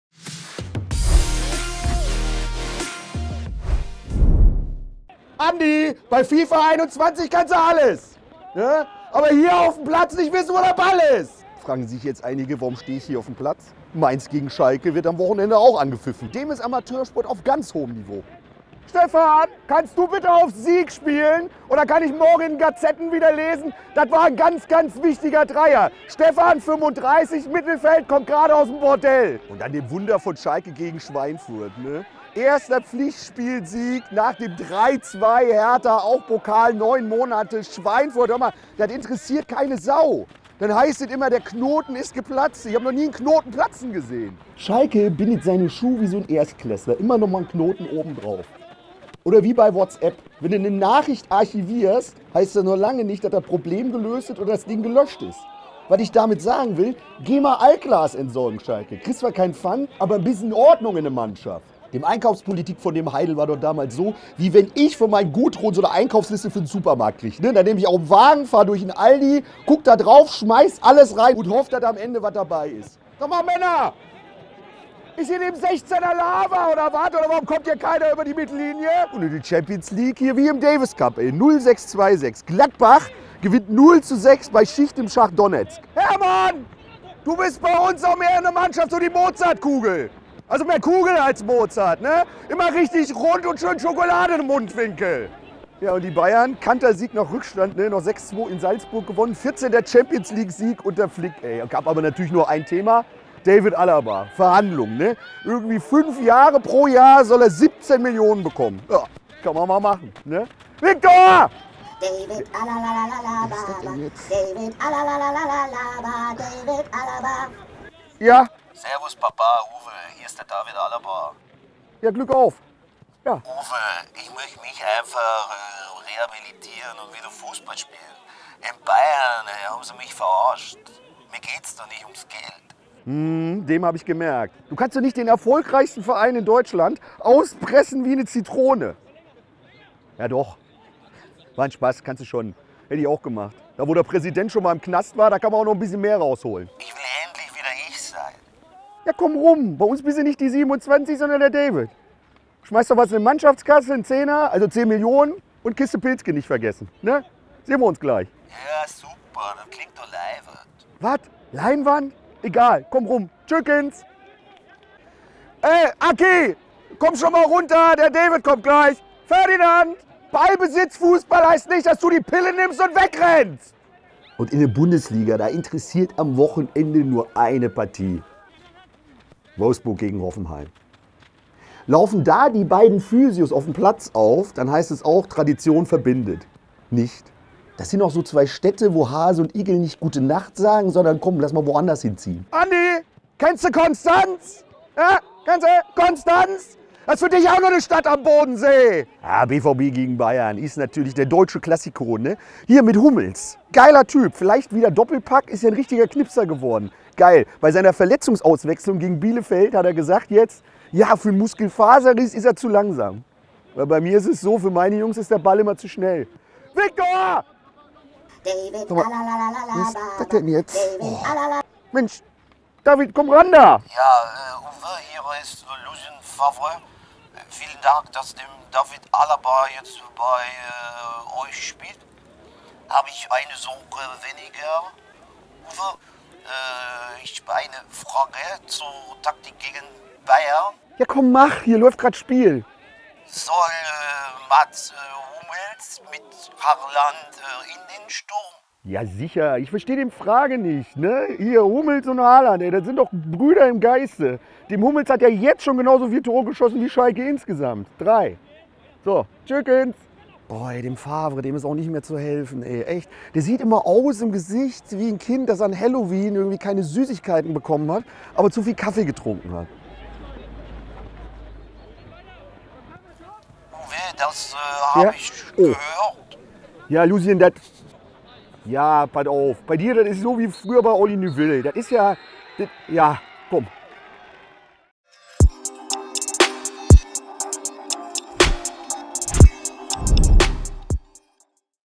Favre & Alaba rufen bei Uwe an, Schalke 04 gewinnt und Hummels in den Sturm. Verrückte Woche!
Haaland und Hummels Brüder im Geiste? Laut Kreisligatrainer Uwe schon. Schließlich hat der Innenverteidiger schon genauso viele Tore wie Schalke erzielt. Auch in der Sache David Alaba kommt Bewegung rein, der Bayern-Star wechselt zu Uwe's Mannschaft in die Kreisliga. Aber auch Gladbach, Schalke und Lucien Favre sind Teil der Wutrede des Kult-Trainers!